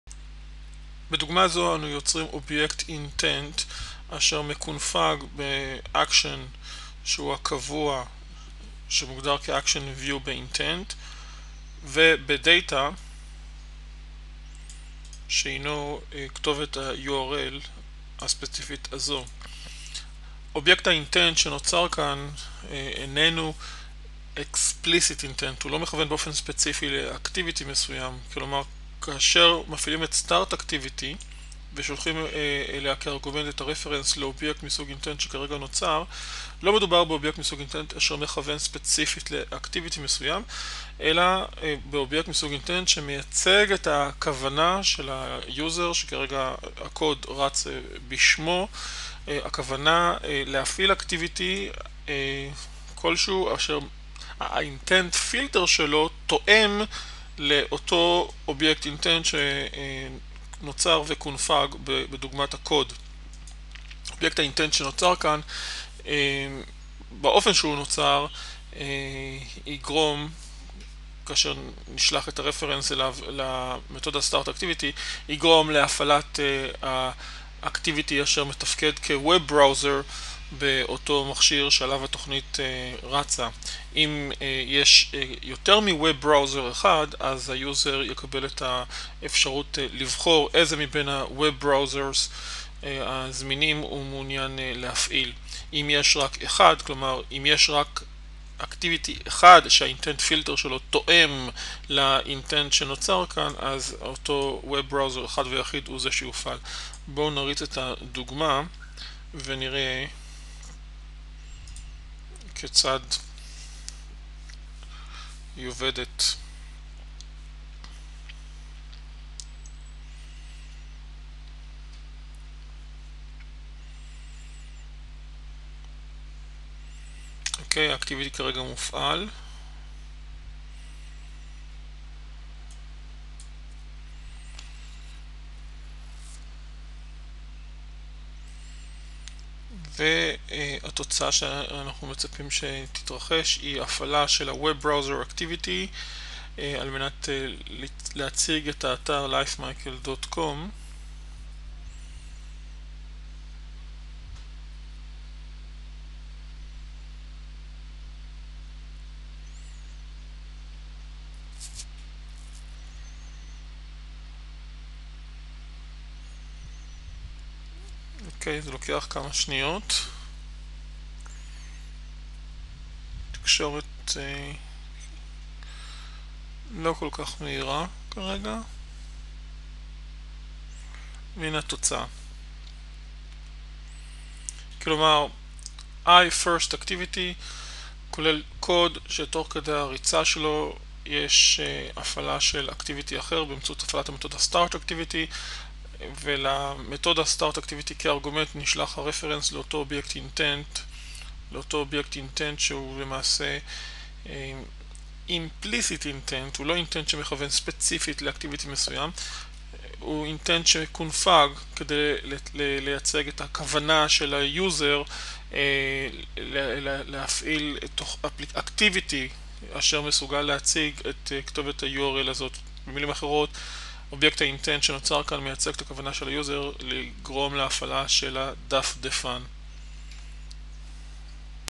דוגמא פשוטה לשימוש ב-Intent
אובייקטים מסוג Intent מייצגים כוונה של ה-user או כוונה של מערכת ההפעלה. דוגמא זו כוללת יצירה של אובייקט מסוג Intent אשר מייצג את הכוונה של ה-user לגרום להפעלה של הדפדפן. כאשר ה-reference לאובייקט זה נשלח כארגומנט למתודה startActivity ה-activity אשר מופעל הוא ה-web browser activity שקיים במכשיר.